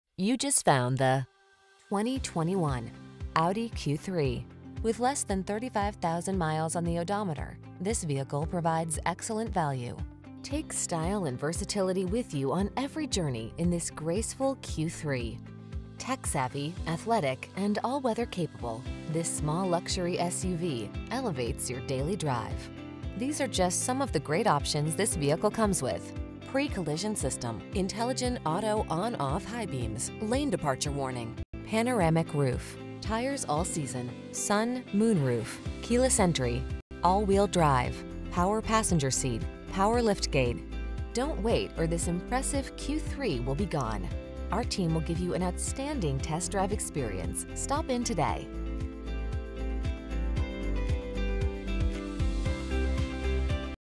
2021 Audi Q3 S line Premium Morton Grove, Glenview, Northbrook, Skokie, Schaumburg IL
Pre-Owned 2021 Audi Q3 S line Premium available at Audi Morton Grove located in 7000 Golf Rd, Morton Grove, IL, 60053. Servicing the Morton Grove, Glenview, Northbrook, Skokie, Schaumburg area.
Please mention the stock PG5695 when you contact the dealership for this vehicle.

You just found the  2021  Audi Q3.  Take style and versatility with you on every journey in this graceful Q3. Tech-savvy, athletic, and all-weather capable, this small luxury SUV elevates your daily drive. These are just some of the great options this vehicle comes with: Pre-Collision System, Intelligent Auto on/off High Beams, Lane Departure Warning, Panoramic Roof, Tires All-Season , Sun/Moonroof, Keyless Entry, All Wheel Drive, Power Passenger Seat, Power Liftgate. Don't wait or this impressive Q3 will be gone. Our team will give you an outstanding test drive experience. Stop in today!